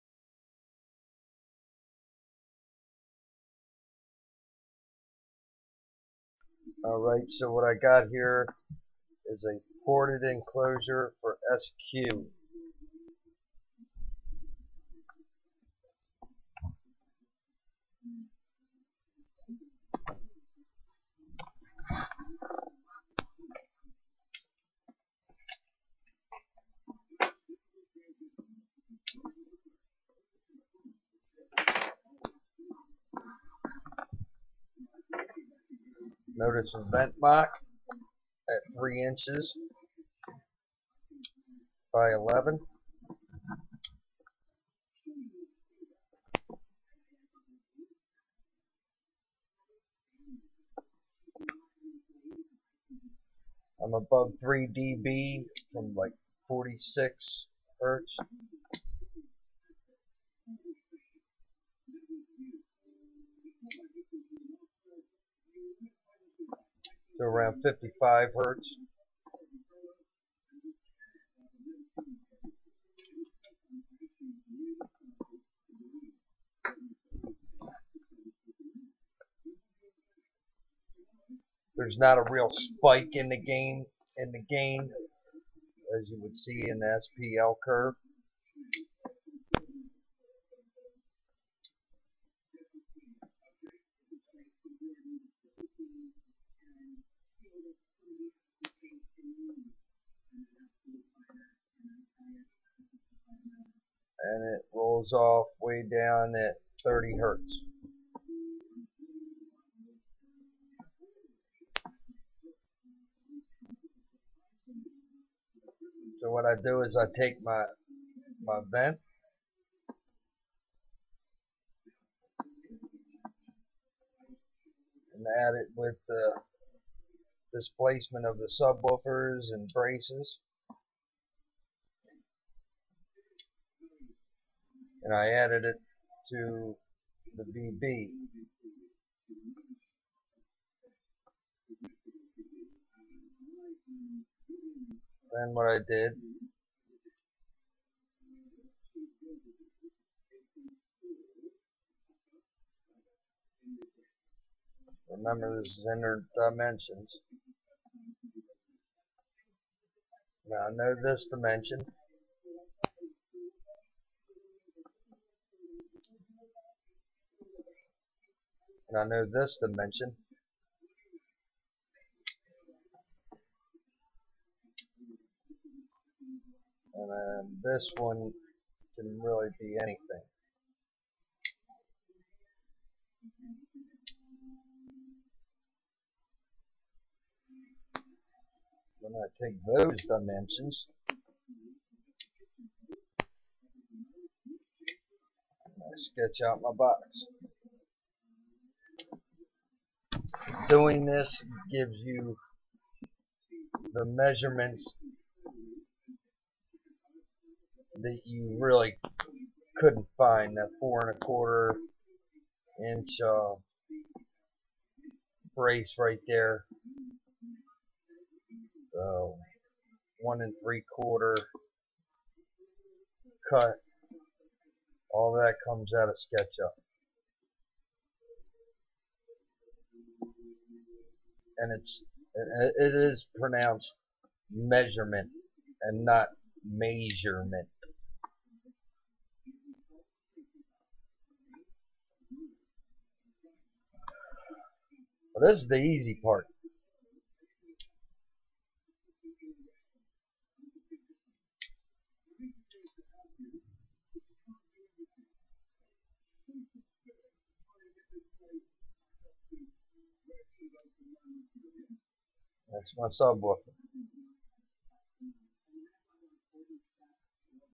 Ported enclosure for sq.
This enclosure is 2 cu. ft net with a port area of 33 sq. in. tuned to 36 hz.